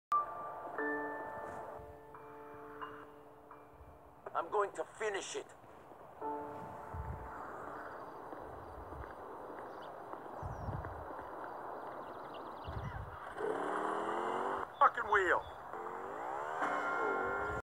Niko Bellic's Revenge (2/10) (2008)
After Niko had a flashback that he found out that Trevor put a C4 in the boat that led to Brian's Death, Niko vows revenge, but trying to do so without getting too angry.